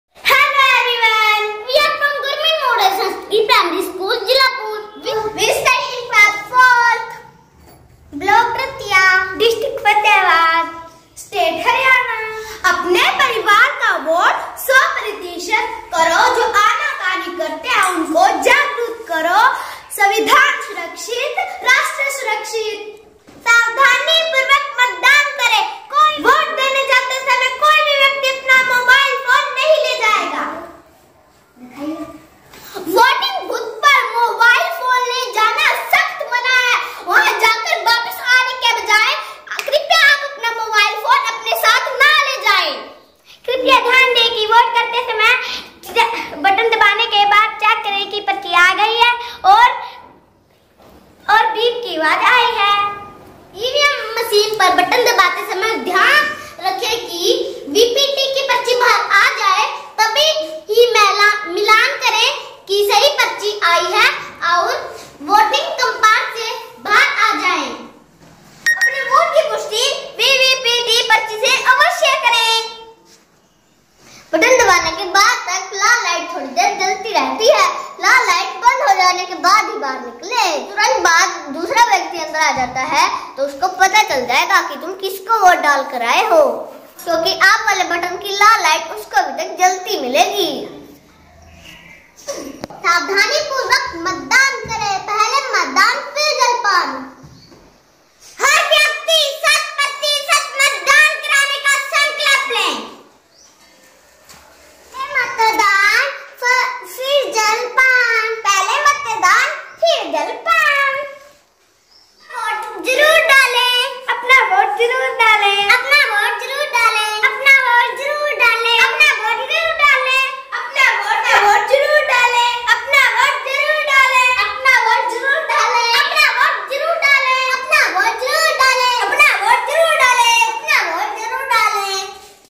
election election2024 electioncommission government india chunav elections2024
पहले मतदान फिर जलपान। समाज में जागरूकता का संदेश देते सरकारी स्कूल हरियाणा से बच्चे
GMSPS JALLOPUR RATIA FATEHABAD HARYANA INDIA